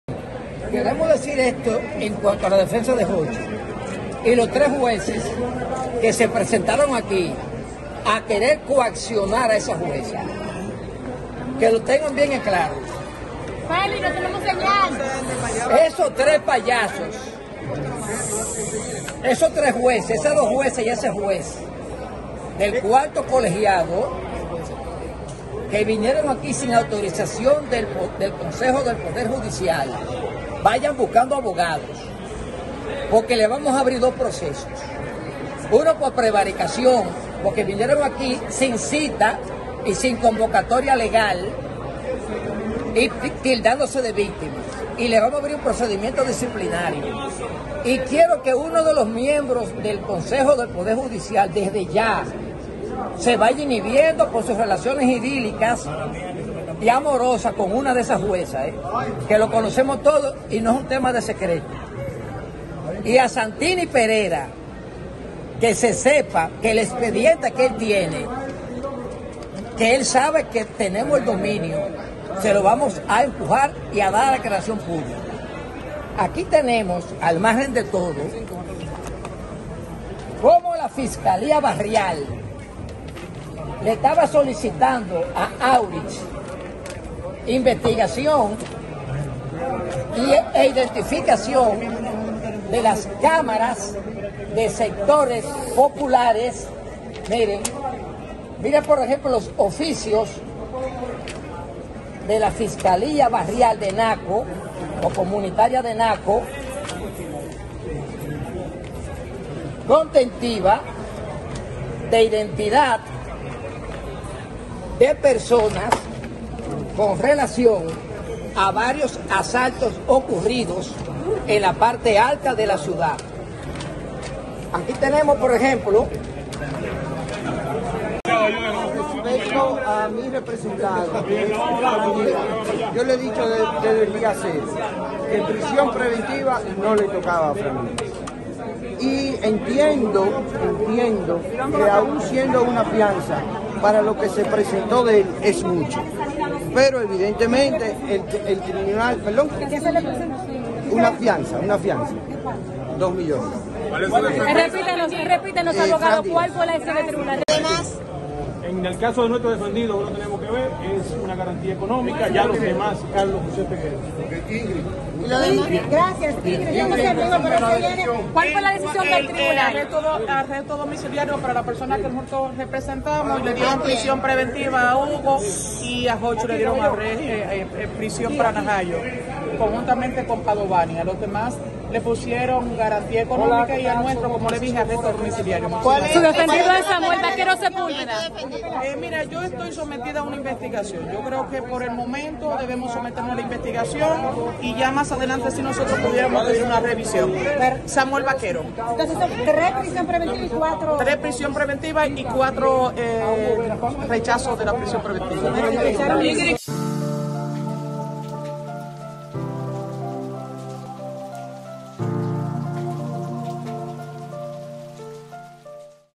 CARLOS BALCARCE LLAMA PAYASOS A JUECES SE PRESENTARON EN AUDIENCIA Y ADVIERTE SERÁN DEMANDADOS
La jueza Fátima Veloz, de la Oficina de Atención Permanente del Distrito Nacional, dictó 18 meses de prisión preventiva contra el exdirector ejecutivo del Instituto Nacional de Transporte y Tránsito Terrestre (Intrant), Hugo Beras; el presidente de Transcore Latam, Jochi Gómez; y Pedro Padovani, exencargado de Control de Tráfico del Intrant, por encabezar un entramado de corrupción en la red semafórica del Gran Santo Domingo.

El caso también fue declarado complejo como lo pidió la Procuraduría Especializada en Persecución de la Corrupción Administrativa (PEPCA).